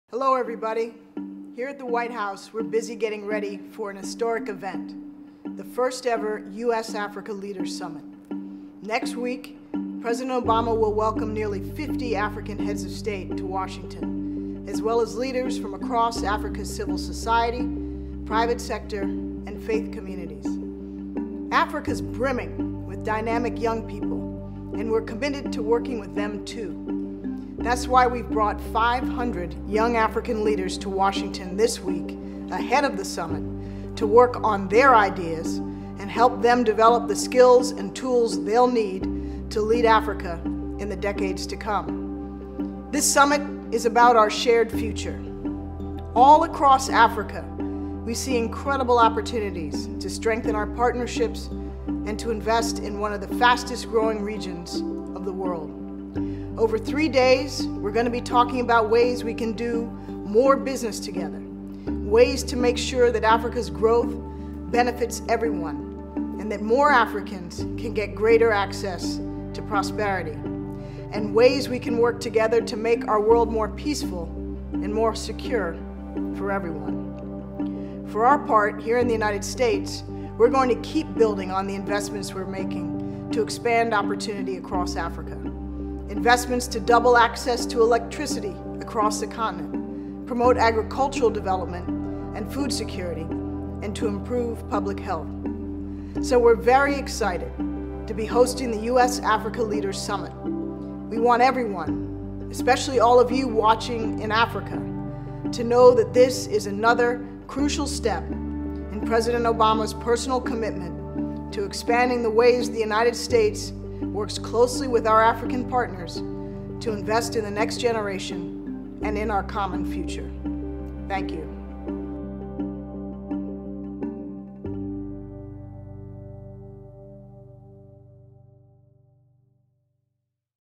Susan E. Rice: Previewing the U.S.- Africa Leaders Summit
United States national security Advisor Susan E. Rice previews the first-ever U.S.-Africa Leaders Summit where President Obama will welcome nearly 50 African heads of state to Washington, as well as leaders from across Africa’s civil society, private sector and faith communities.